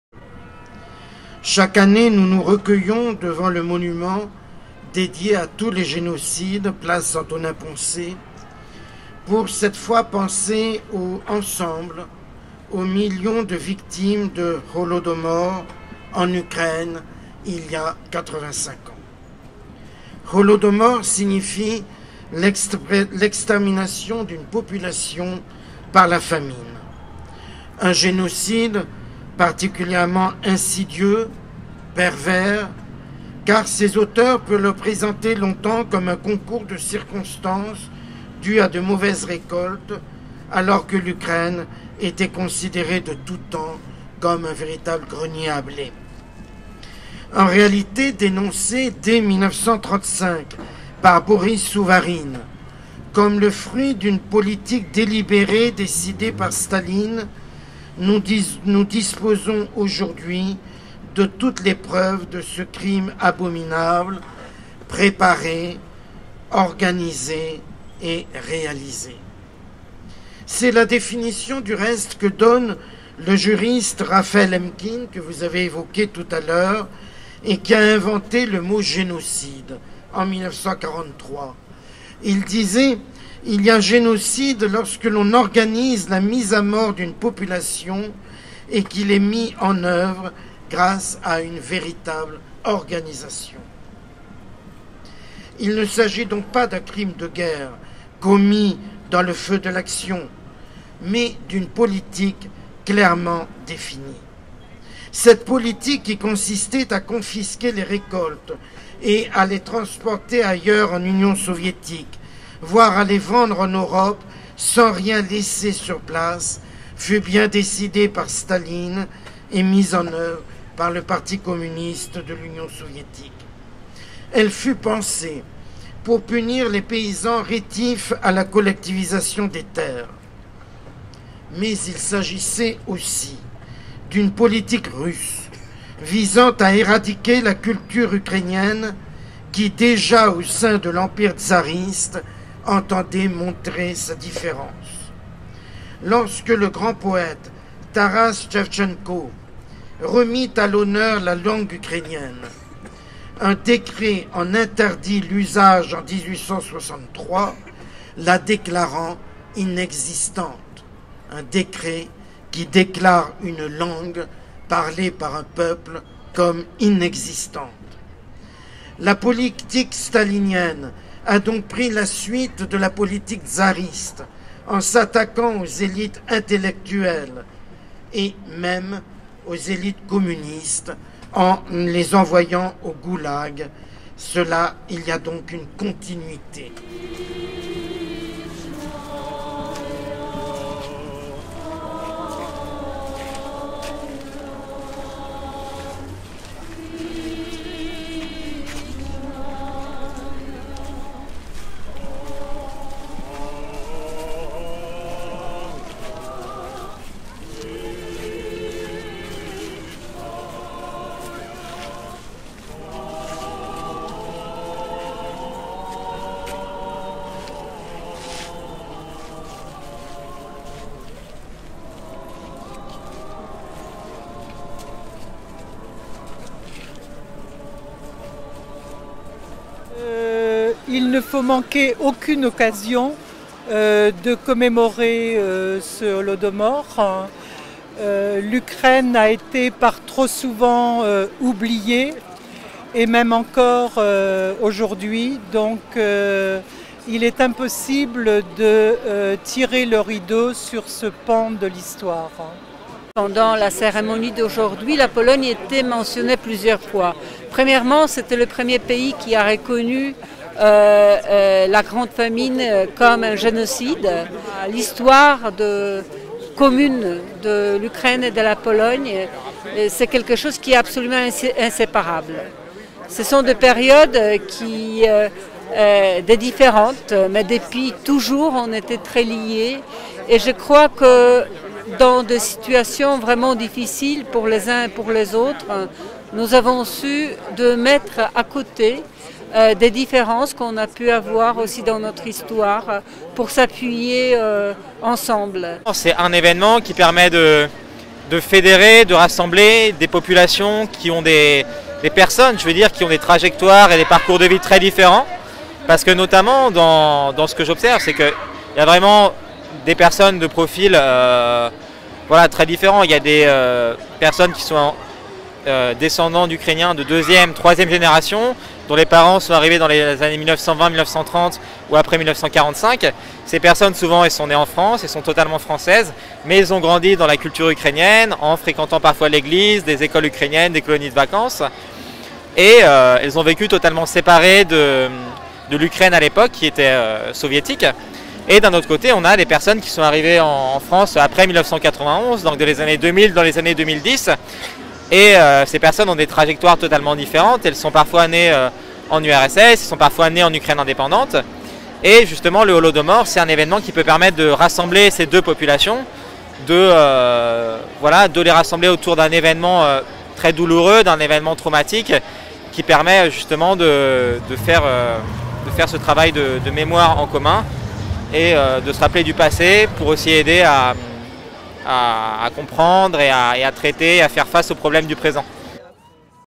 Commémoration du Holodomor au Mémorial lyonnais de tous les génocides
24/11/2018 : la commémoration du 85 anniversaire du Holodomor au Mémorial lyonnais de tous les génocides. Écoutez le discours de Jean-Dominique Durand, adjoint au Maire de Lyon, ainsi que les commentaires d'Annick Bilobran, président de l'Advule, Joanna Kozińska-Frybes, Consul Général de la République de Pologne à Lyon, Hervé Amiot, doctorant en géographie (Université Paris 1), étudie la diaspora ukrainienne.
Merci à l’organisateur de l'evenement, le Comite Ukraine 33.

Pour écoutez plus de prises de paroles rdv à